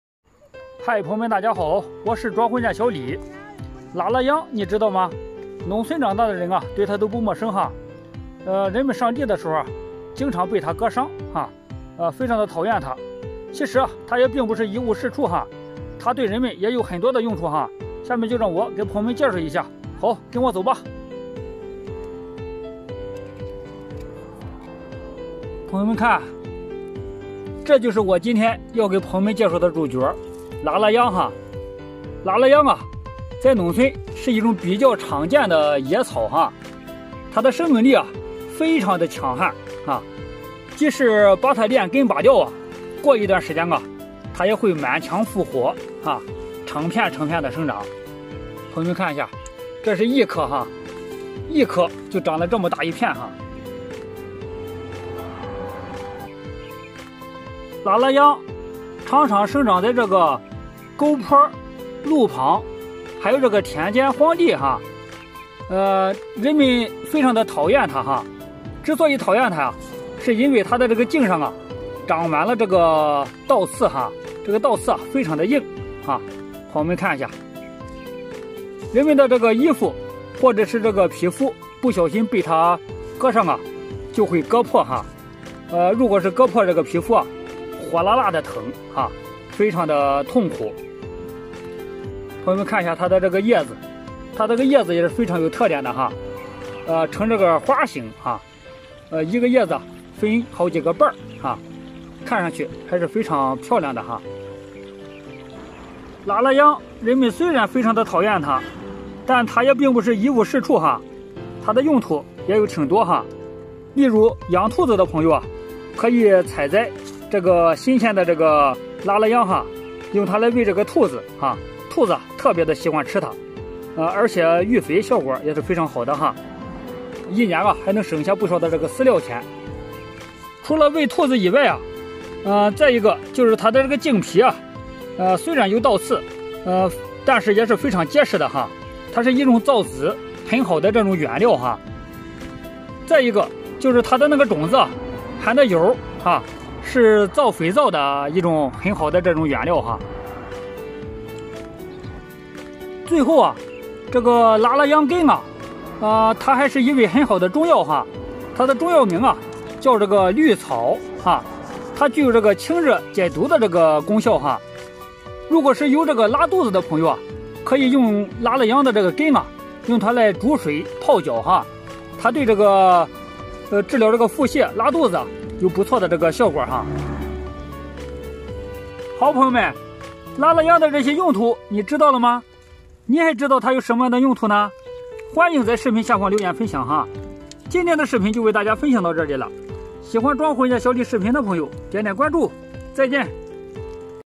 农村令人讨厌的拉拉秧，原来用处有这么多，你还不知道吧
个人描述：乡村辉哥，每天记录身边的美好生活